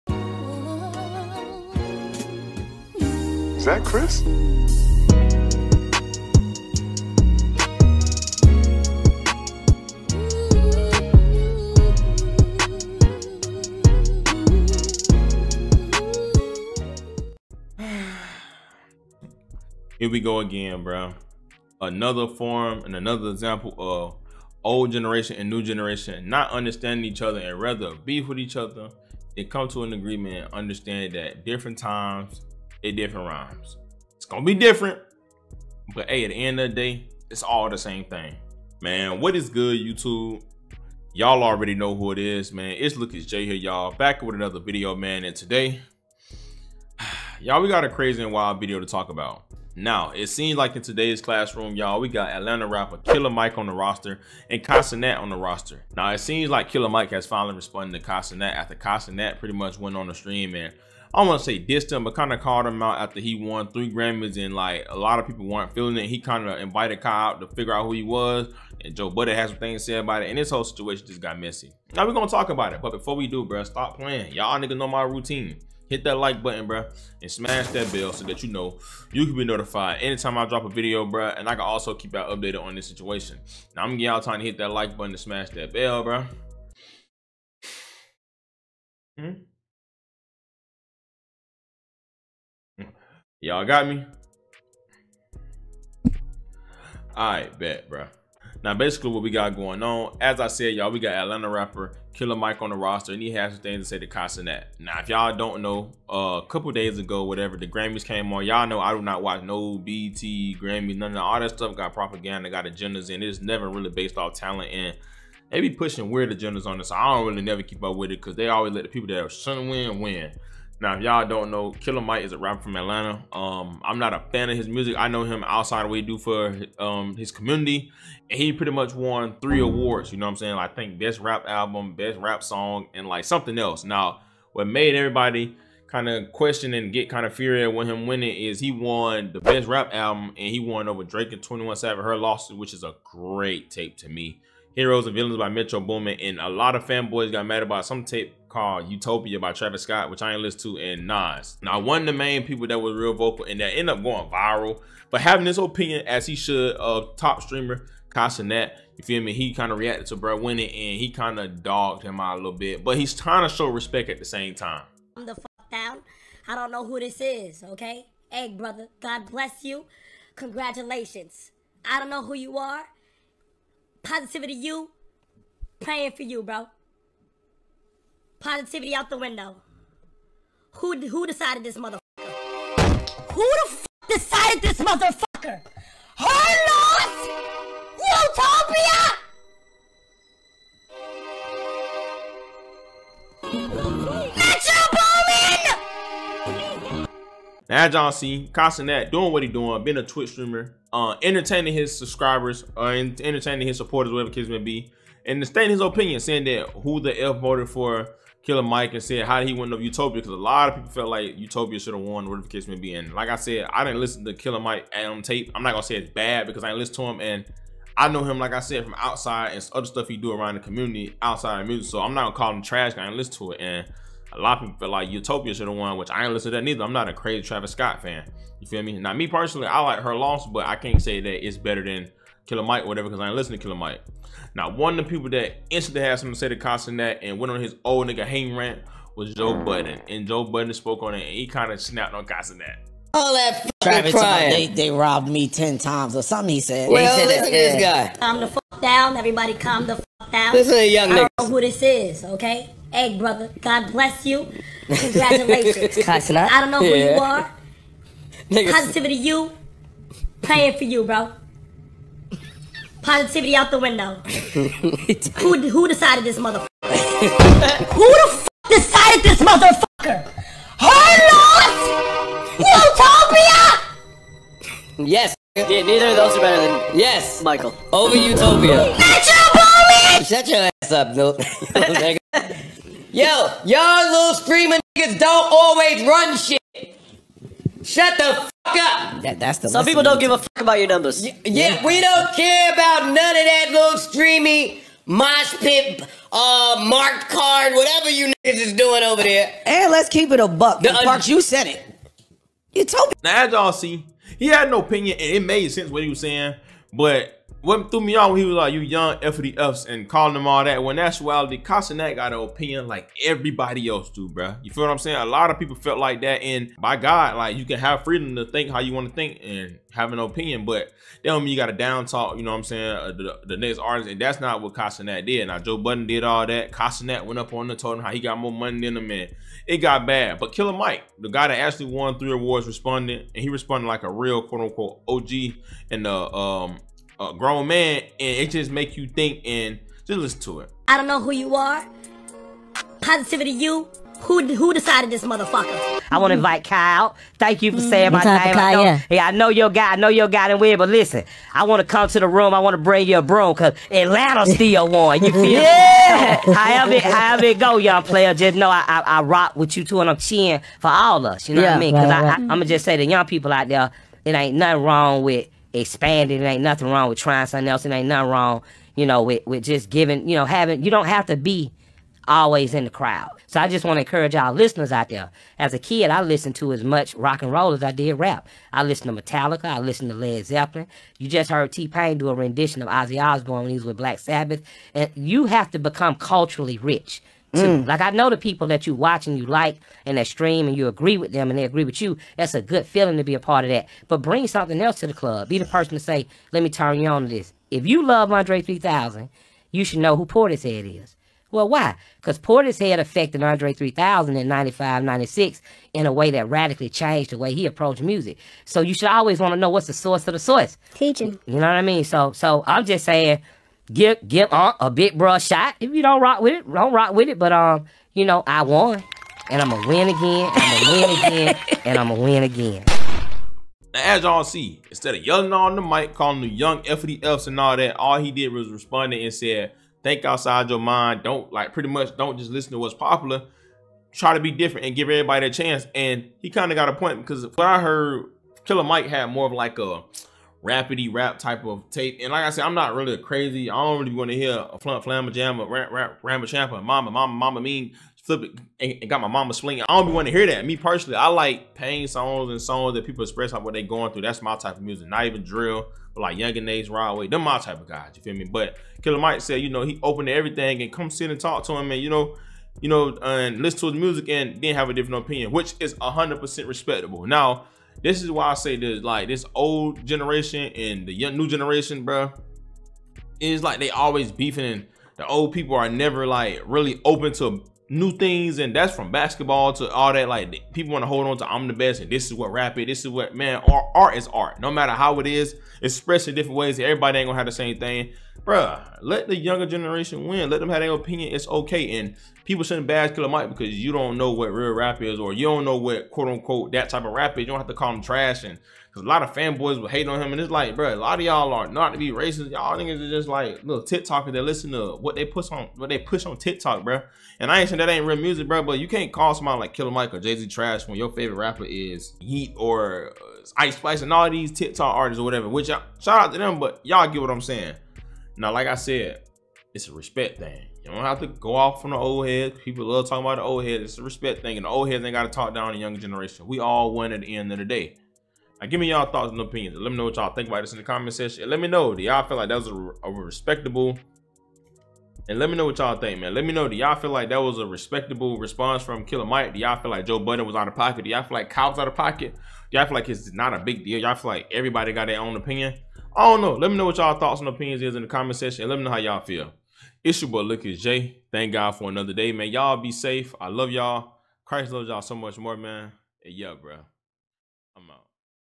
Killer Mike Sends Kai Cenat A Message After D!ssing Him Over Winning A Grammy!?
Wow.

Playboi Carti Kick 21 Savage Adin Ross Charleston White Kai Cenat